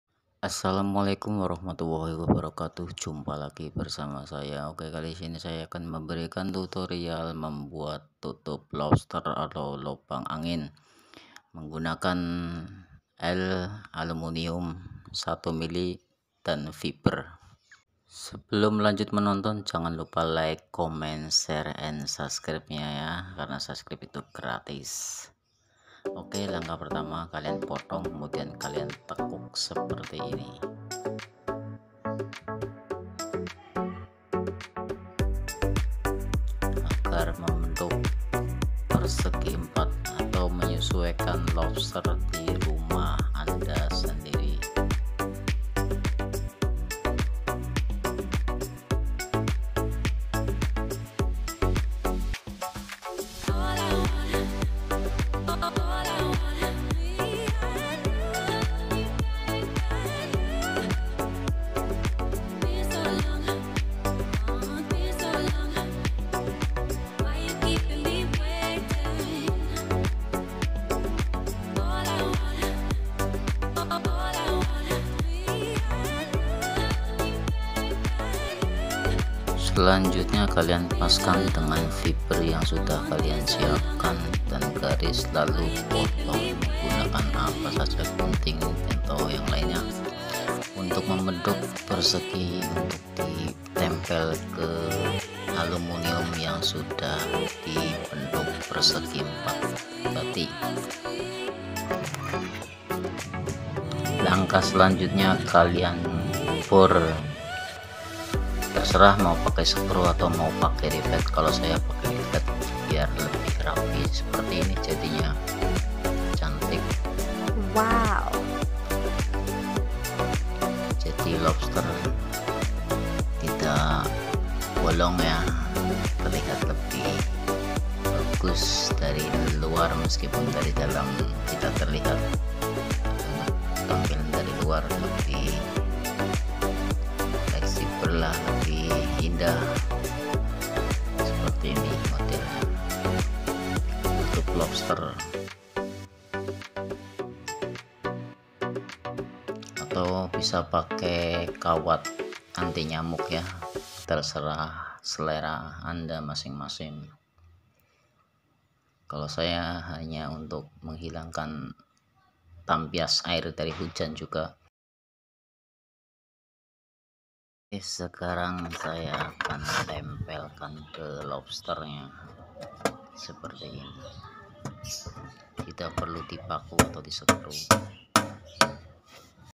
cara Membuat Tutup Ventilasi Ruangan ber AC minimalis bisa lepas pasang
cara membuat tutup ventilasi ruangan minimalis atau biasa di sebut lubang angin/lobster

mudah cukup dengan Lis siku dan fiber dan paku rivet

ini cara yg saya buat untuk membuat tutup ventilasi udara, jika Anda ingin menggunakan holo Bajaringan/bahan lainnya juga bisa